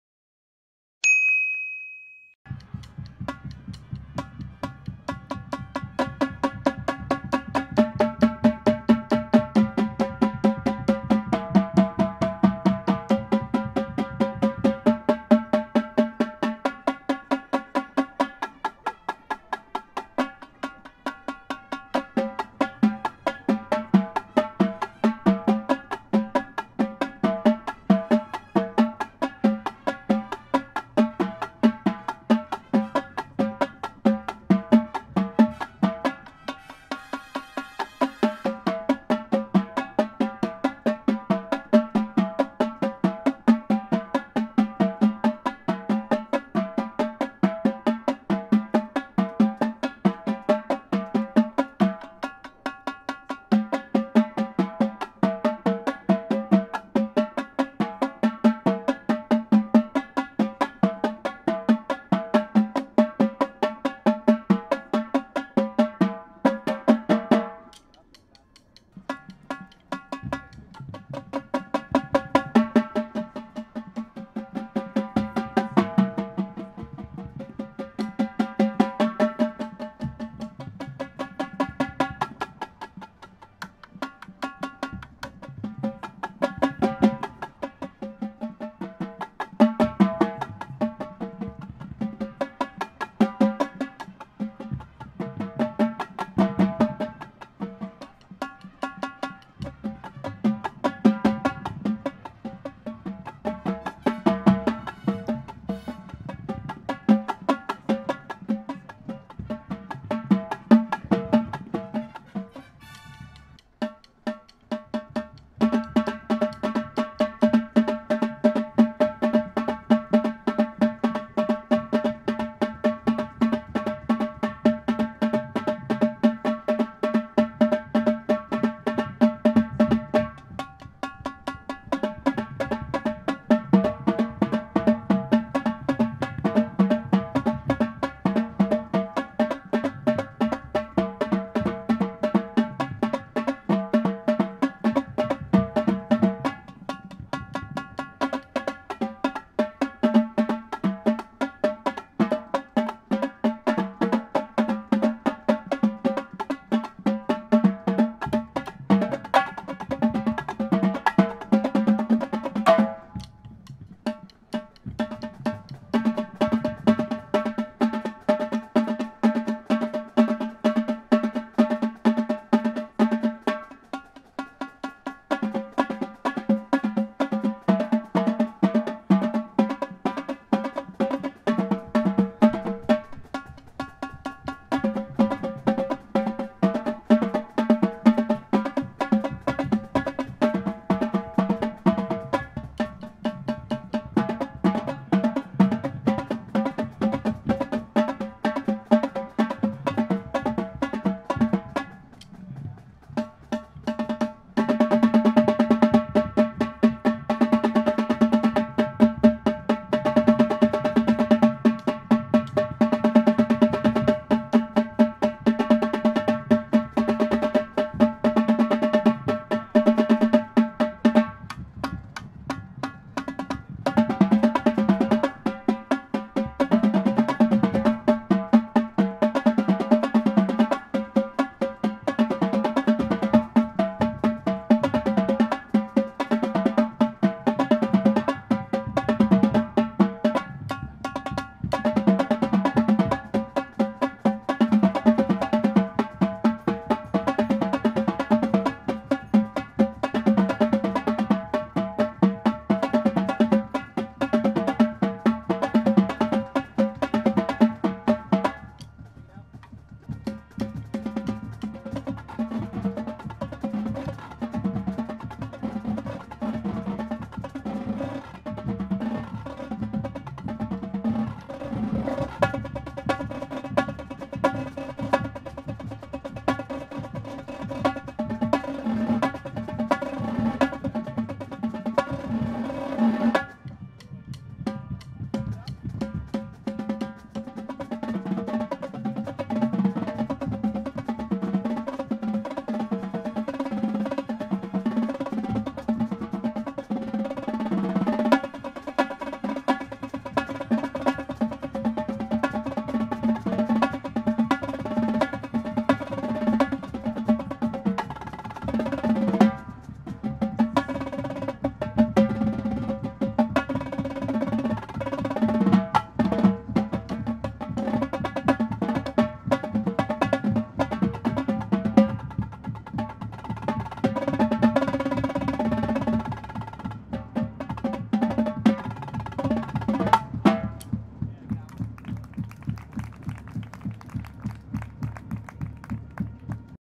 Phantom Regiment Quads Exercises 2024 || DCI Prelims
The Regiment Quads go through their warm up routine in the lot at DCI Prelims 8/8/24

Camera: Sony ZV-E1
Lens: Zeiss 55mm f/1.8
Audio: Tascam DR-05x